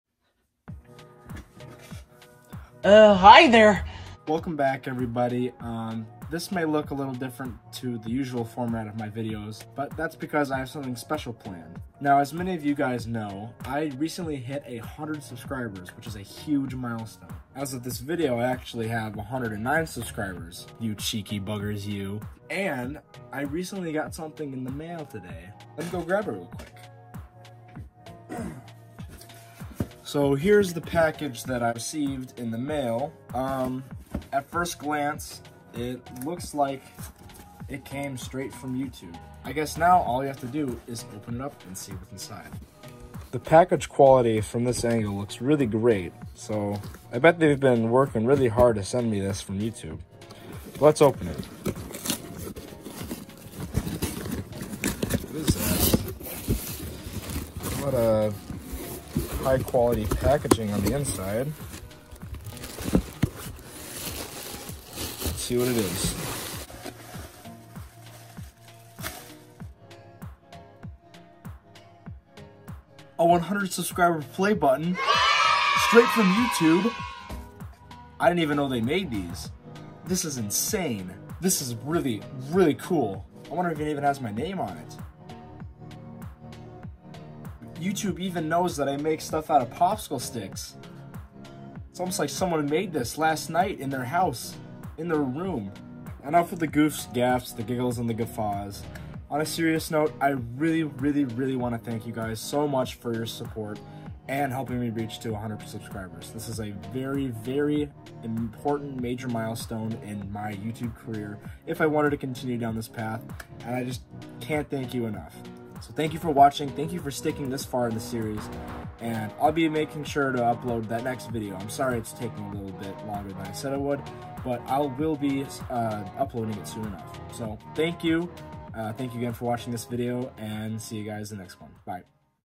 Popsicle stick house | video 11.5 YOUTUBE SENT ME SOMETHING IN THE MAIL??? (100 sub special)
as I said in my post, I can't thank you guys enough for this major milestone. I would like to personally thank each and every one of you for taking the time to watch my videos, to like them, and to subscribe to such a small channel.